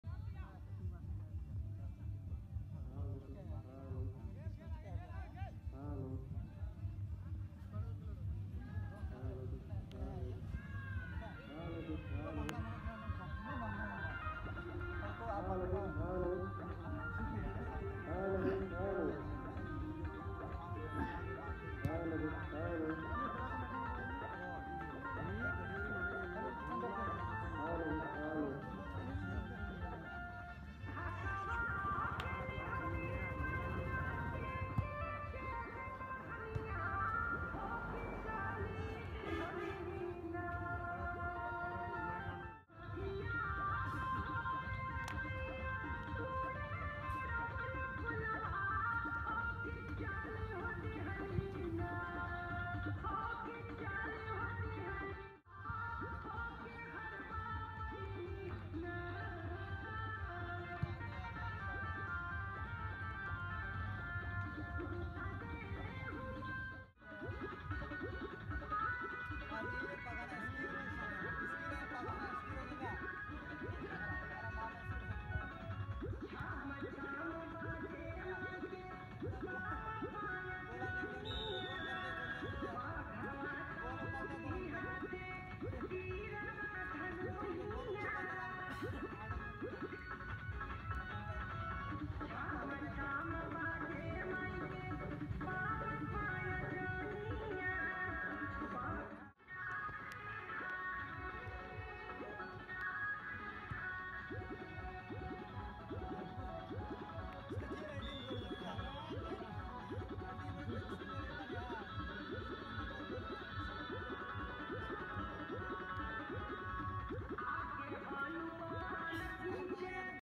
Pritam और Sachin Ka Shandar Goal Second Round Match Me // At :- Sugathan Poreyahat Godda
Pritam और Sachin Ka Shandar Goal Second Round Match Me // At :- Sugathan Poreyahat Godda @RAJESHMURMU8292